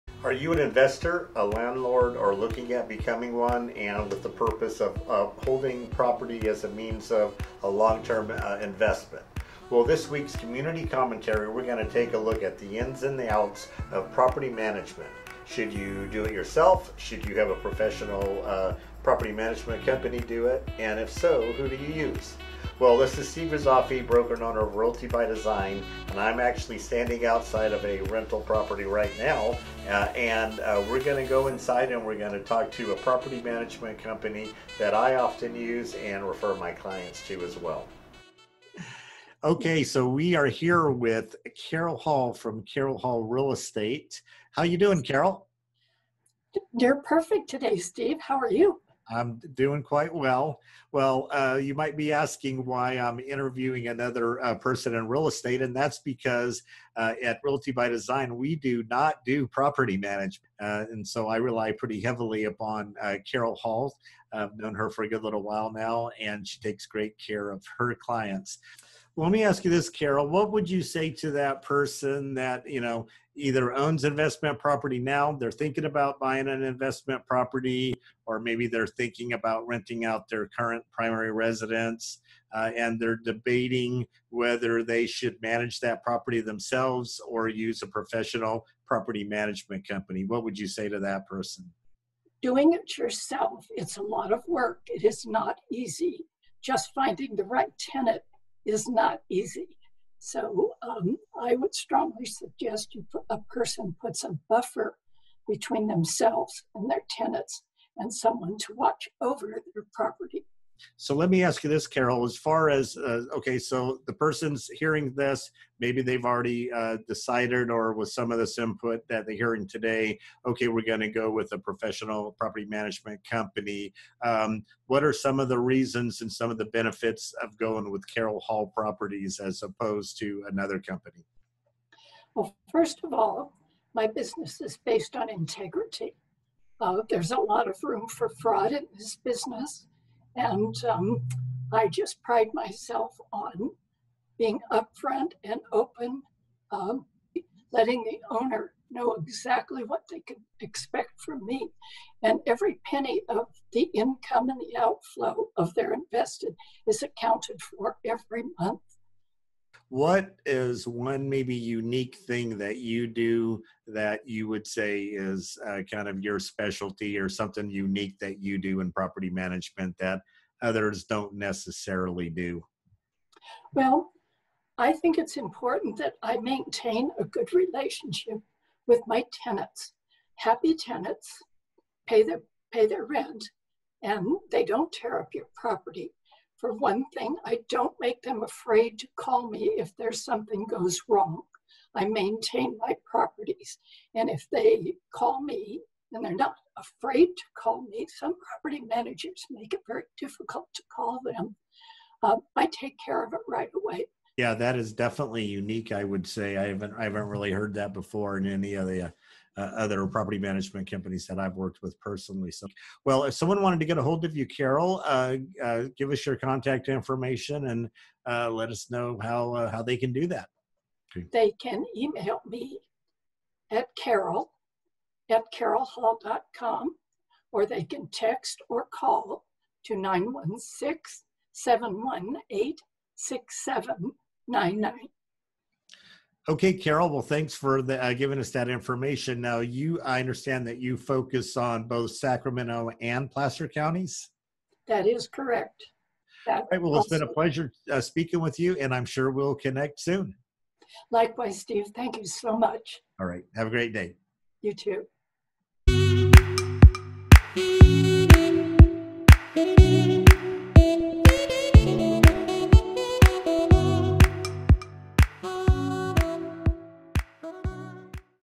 Real Estate Buy and Hold - Property Management - Placer County California
In this community commentary, we look at the in's and out's of managing a rental property.